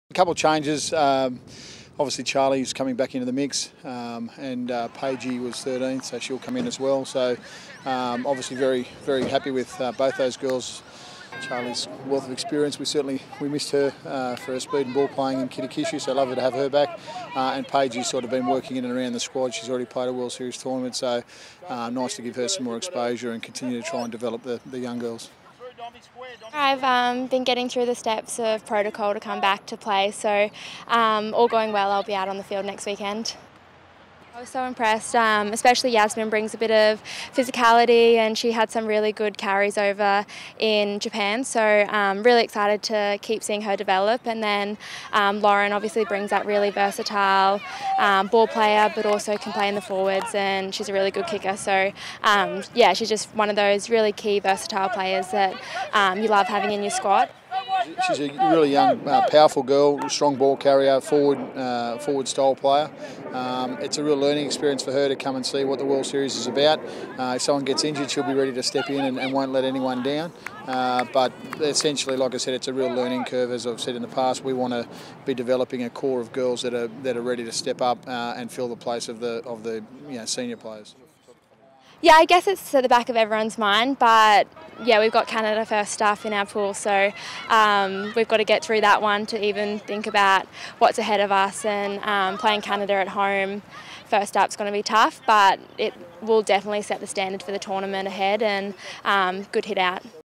Caslick returns, rookies retain spots
Aussie superstar Charlotte Caslick will make her return next weekend in Canada after recovering from a head knock picked up at the Commonwealth Games.  She joins a relatively young team as they look to take a step further in securing the World Series